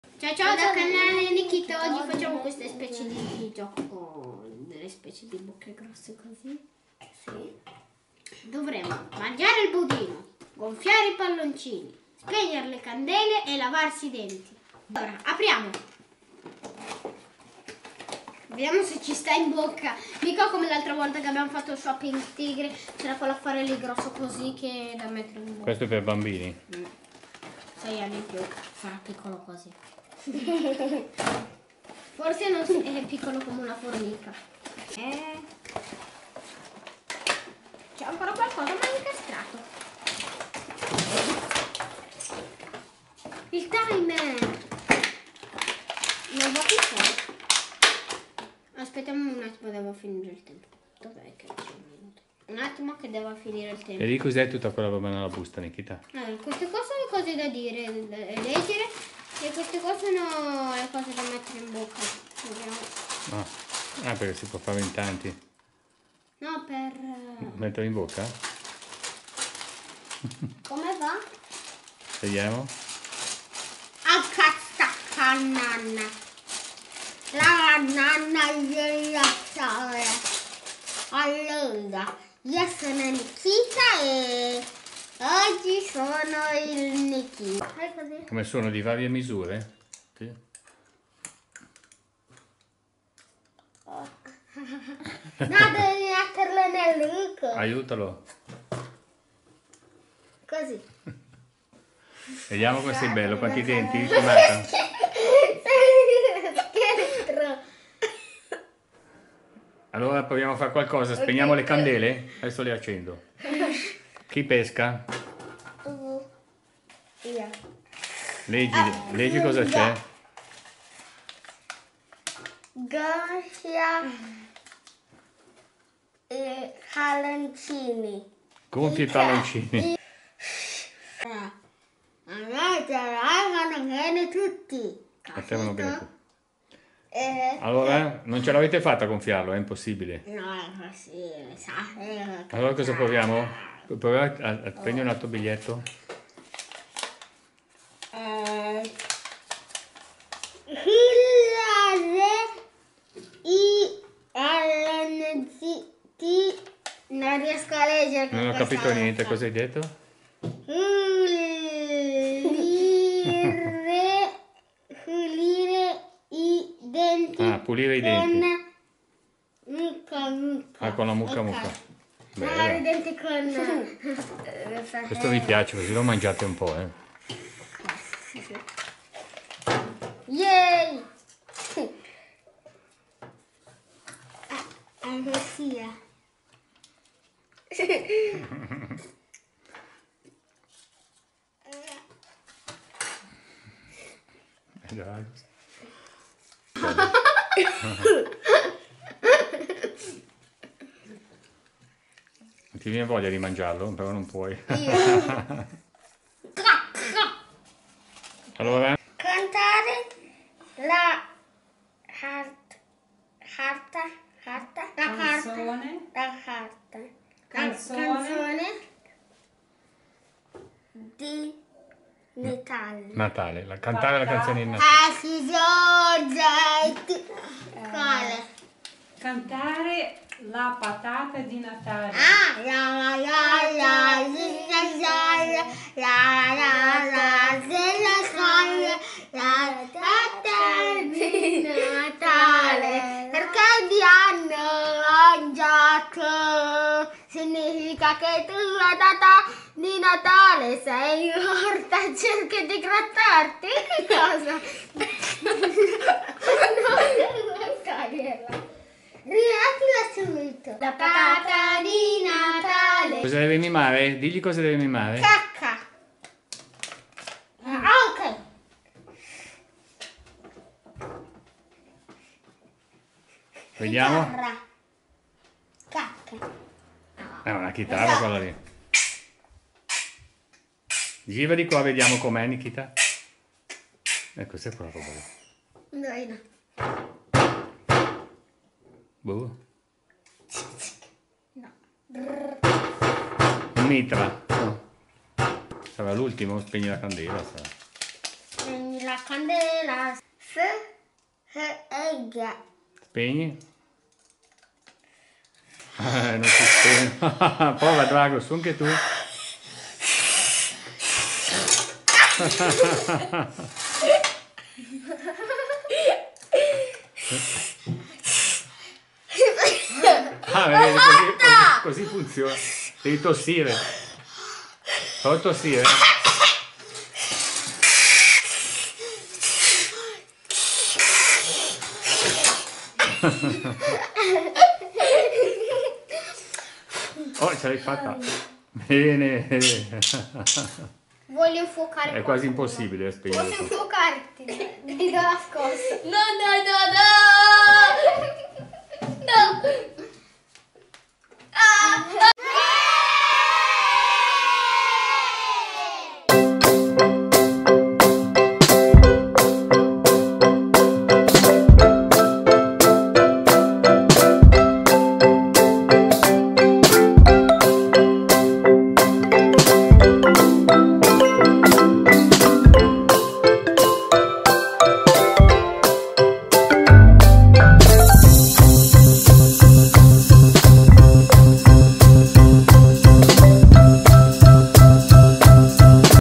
THE MOUTH CHALLENGE ita - MOUTHGUARD SFIDA DISGUSTOSA – Speak out Apertura gioco Canale Nikita
THE MOUTH CHALLENGE ita - MOUTHGUARD SFIDA DISGUSTOSA – Speak out Canale Nikita
Oggi monello Nikita gioca con il suo amico Dragos a Mouthguard Challenge ita . Il gioco si chiama Mouthguard Game “Say it .. don’t spray it” (versione per famiglia , analogo di Speak out , ma adatto per i bambini ) …Apriamo insieme questo gioco , consigliamo per bimbi e ragazzi , sempre divertente giocare con amici e la famiglia

Today we're doing the MOUTHGUARD CHALLENGE using the Mouthguard Game “Say it .. don’t spray it”
About this game:
Get ready to be doubled over with laughter with this ridiculous mouthpiece challenge game! The Mouthguard Game brings friends and family together for laugh-out-loud fun as players try to say different phrases while wearing a mouthpiece that won't let them shut their mouth. In this game, players draw from the deck and read the phrase as best as they can, but it's not so easy to do when the mouthpiece is hindering them from forming words correctly. The timer counts down as players try to say phrases such as "he's my stealthy pet ferret name Garrett", "slow down, you careless clown", or "pelicans love pollywog falafels." The laughter continues as players try to decipher what in the world their teammate is trying to say, and must correctly guess the phrase in order to earn the card. The team with the most cards at the end of the game wins. The Mouthguard Game includes 100 double-sided cards, 10 mouthpieces, and a timer.

The game is recommended for ages 6+ but with the help of parental supervision and a smaller cheek retractor, younger kids are able to join in on the fun as well. We might have to play this for Family Game Night! I think this game used to be called Watch Ya Mouth o Speak out ( of Hasbro Gaming).